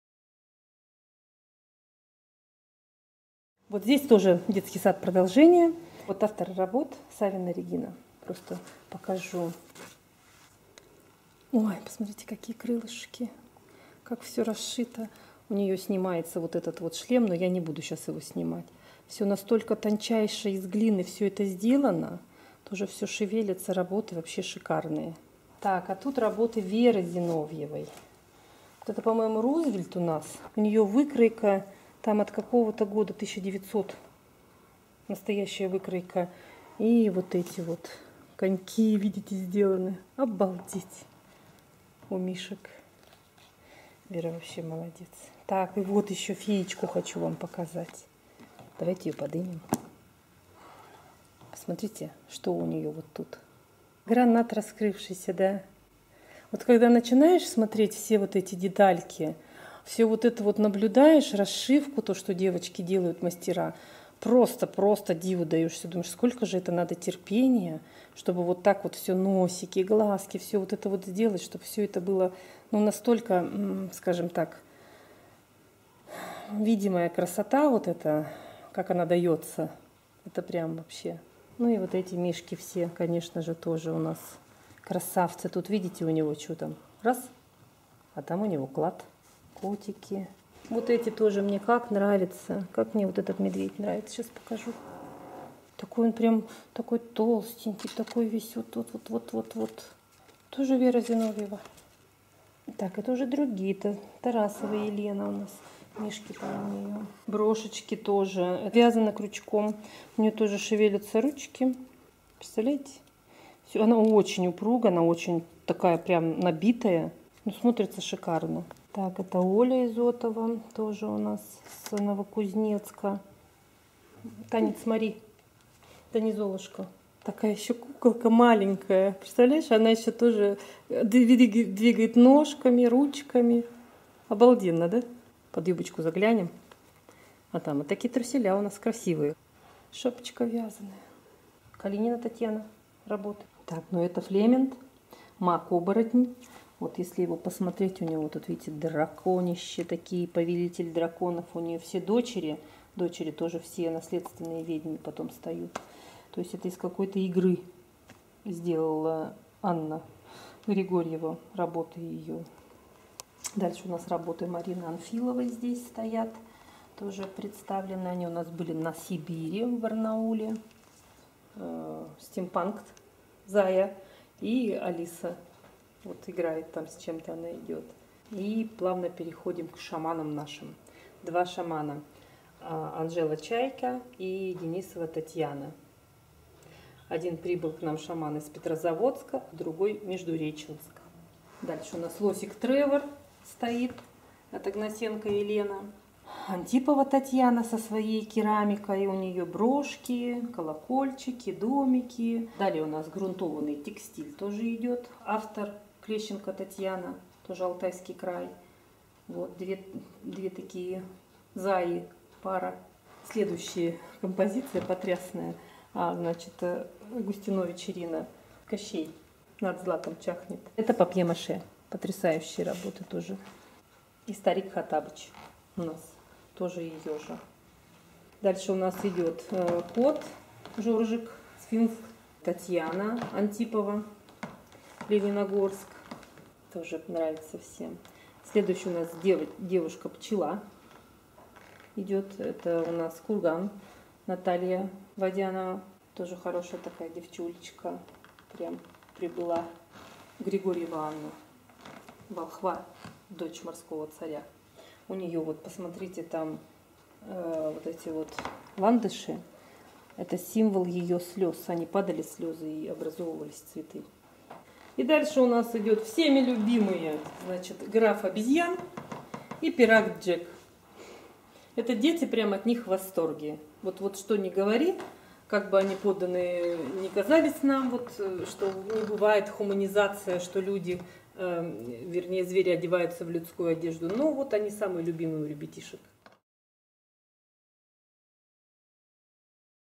Выставка авторских кукол «Здесь оживают сказочные сны». Часть 4
Четвертая часть видеообзора с выставки авторских кукол «Здесь оживают сказочные сны» в г. Заринске (Алтайский край). Выставка собрала 43 мастера из 37 городов России, которые предоставили свои удивительные работы в разных техниках: лепка из полимерной глины, ватная игрушка, тряпичная кукла, грунтованный текстиль, смешанные техники.

Творческая мастерская Delki.ru, 2023 г.